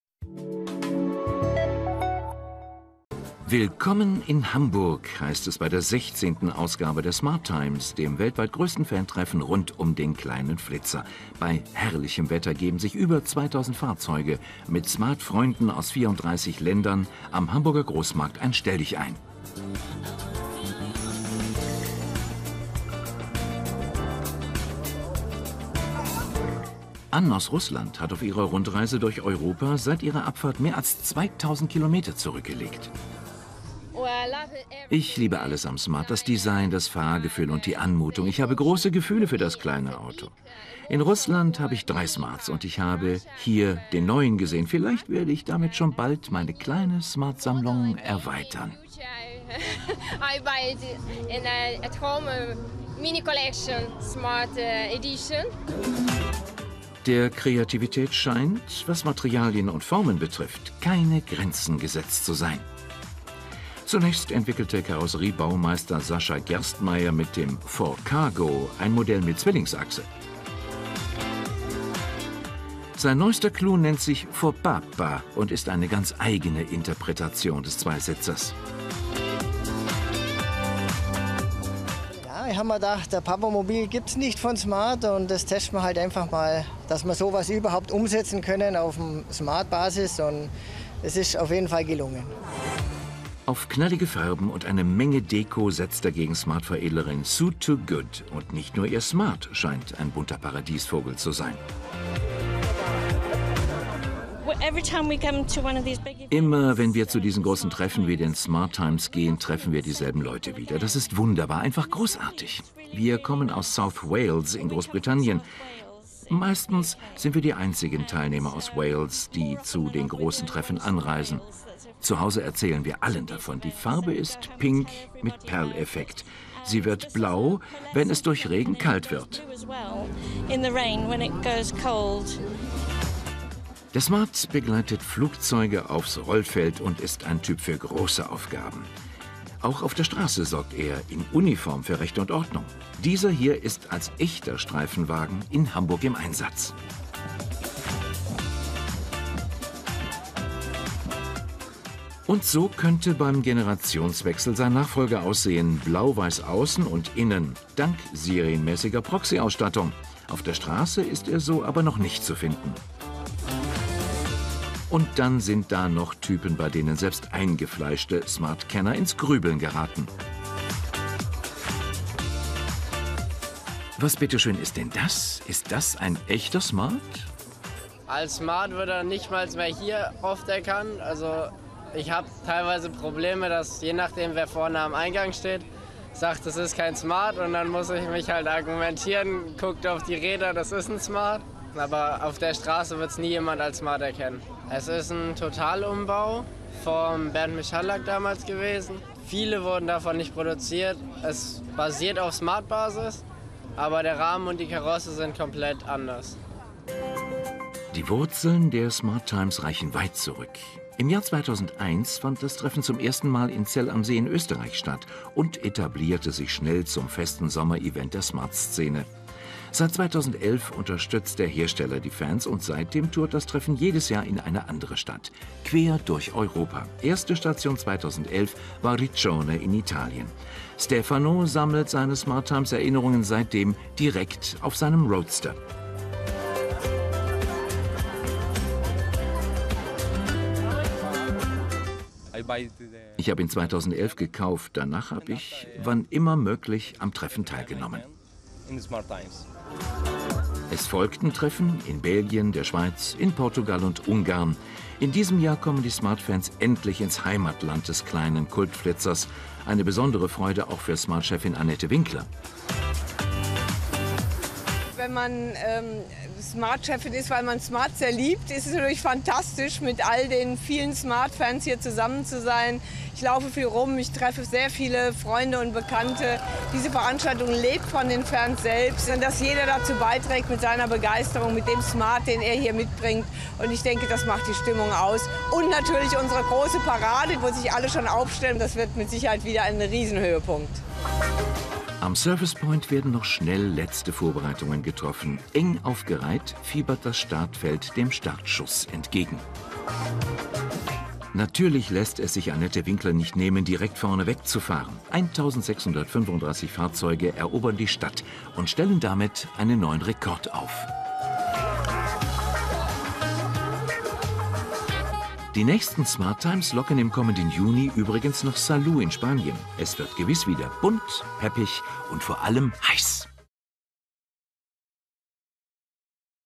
Besuch in Hamburg: die Smart Times 2016 | Motor mobil
Smart-Fans aus vielen Ländern treffen sich jährlich mit ihren Fahrzeugen zum Austausch. 2016 fand dieses Treffen in Hamburg statt.